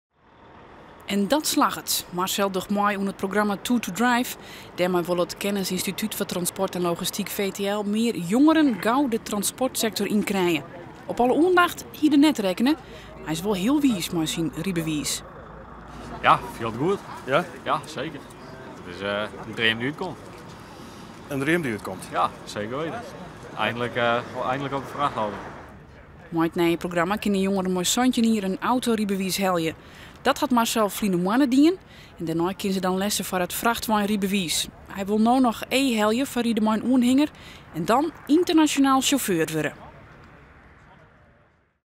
Marcel (18) is de jongste frachtweinsjauffeur fan Nederlân
De 18-jierrige Marcel Postma fan Frjentsjer hat tongersdei yn Ljouwert syn frachtwein-rydbewiis helle. Dat is bysûnder, want Marcel is noch mar trije dagen 18. Hy is no de jongste Nederlander dy't op in frachtwein ride mei. Marcel hat gebrûk makke fan it programma 2toDrive, wêrby't je al op je 17de lessen op in frachtwein krije kinne.
Njonken it diploma krige Marcel ek blommen en kado's fan Transport & Logistiek Nederlân. Dizze organisaasje is wiis mei jonges as Marcel, omdat de kommende jierren in tekoart oan sjauffeurs ferwacht wurdt.Marcel hat de leafde foar de frachtauto net fan in frjemd, want syn heit, omke en pake binne ek frachtweinsjauffeur. Ynternasjonaal ride, fynt Marcel is moaiste wat der is.